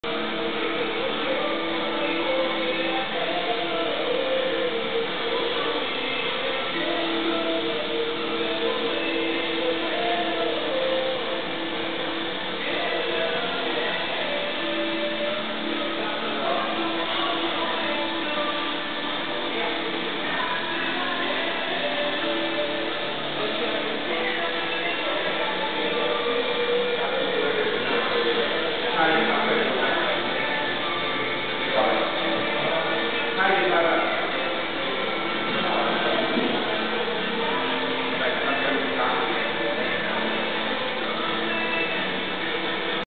Malowanie kręgielni
Malowanie graffiti w kręgielni, aranżacja ścian w kręgielni, malowanie obrazu na ścianie, mural ścienny. Obraz malowany na ścianie metodą natryskową za pomocą areografu. Tematyka pracy jest związana z morzem a  dokładnie piractwem i korsarstwem. Celem malowanie jest podniesienie estetyki otoczenia kręgielni. więcej zdjęć na: